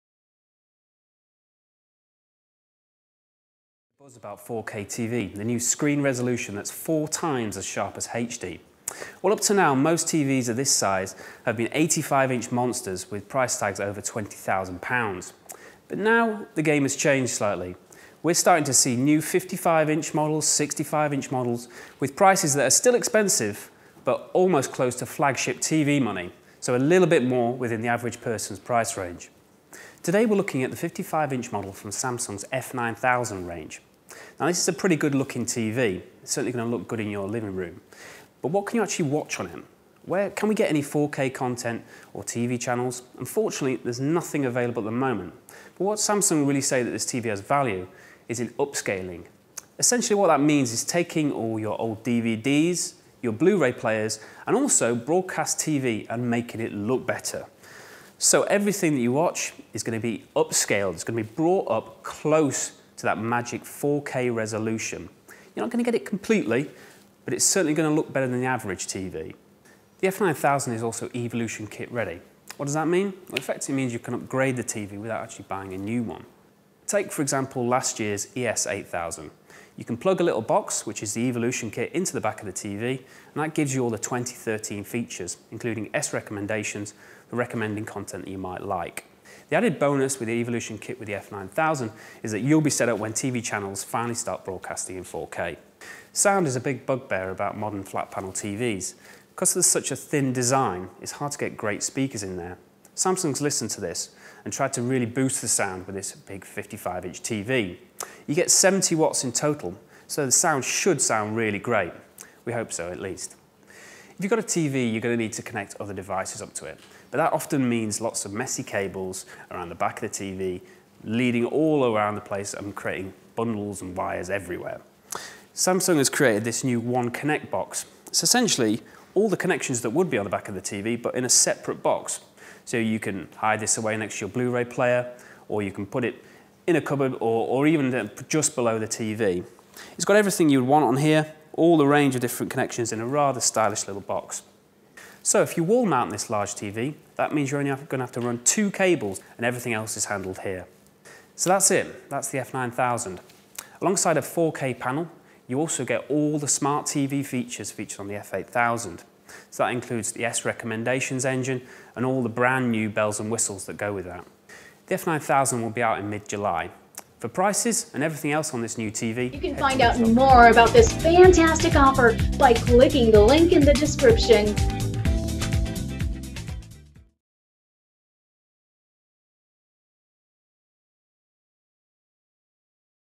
Samsung 65-Inch review ( 4 out of 5 stars )
Samsung UN65F8000 65-Inch 1080p received 4 and 1/2 out of 5 stars in it's review on amazon.